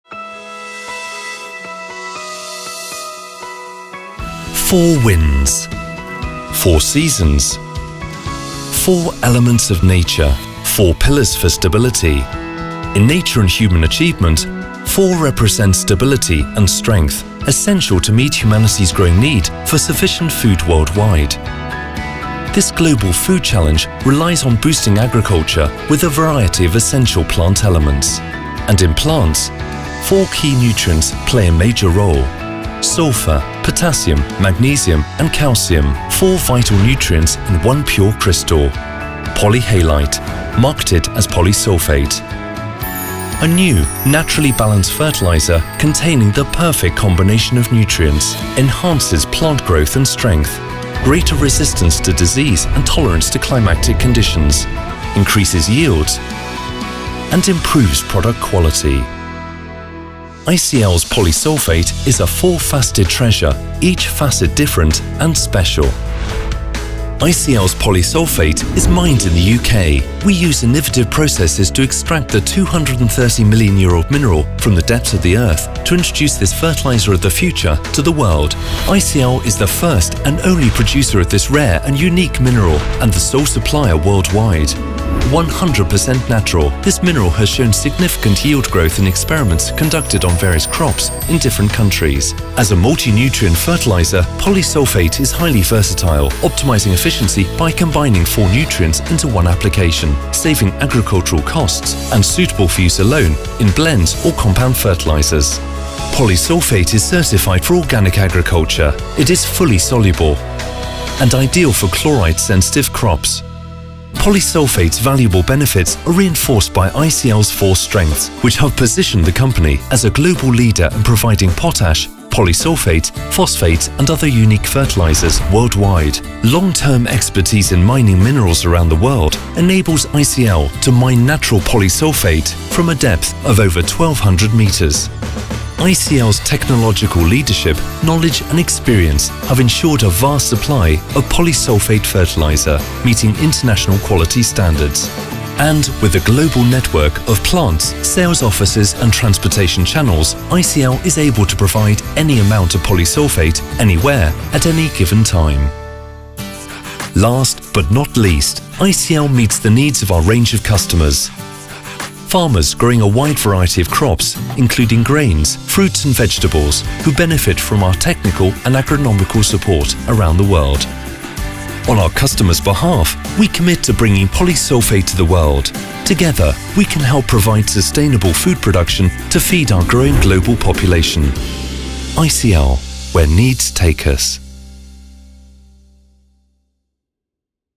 Introducing Polysulphate® (UK)
A new tool for organic farming.

Polyhalite is a new organic fertilizer with readily available sulphur plus potassium, magnesium and calcium. It has a number of key benefits, ensuring increased crop yields and crop quality for farmers. Polyhalite, marketed as Polysulphate, is manufactured and sold only by ICL Fertilizers. This unique fertilizer is also suitable for chloride-sensitive crops.